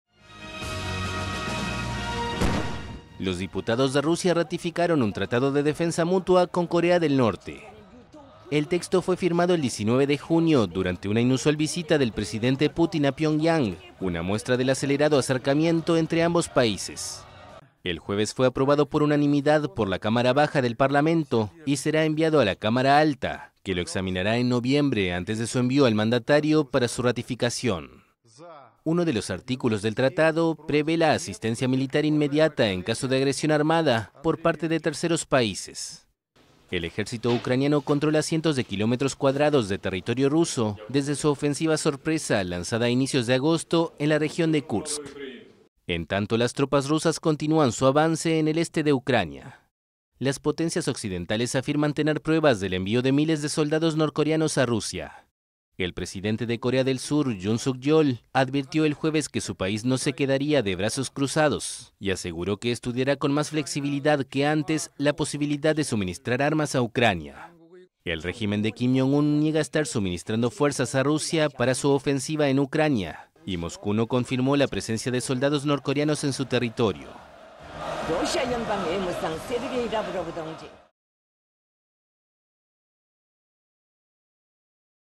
RUSIA: Diputados rusos ratifican por unanimidad un tratado de defensa mutua con Corea del Norte
El texto fue firmado el 19 de junio durante una inusual visita del presidente Putin a Pyongyang, una muestra del acelerado acercamiento entre ambos países.
El jueves fue aprobado por unanimidad por la Cámara Baja del Parlamento, y será enviado a la Cámara Alta, que lo examinará en noviembre antes de su envío al mandatario para su ratificación.
Uno de los artículos del tratado prevé la asistencia militar inmediata en caso de agresión armada por parte de terceros países.
El ejército ucraniano controla cientos de kilómetros cuadrados de territorio ruso desde su ofensiva sorpresa lanzada a inicios de agosto en la región de Kursk.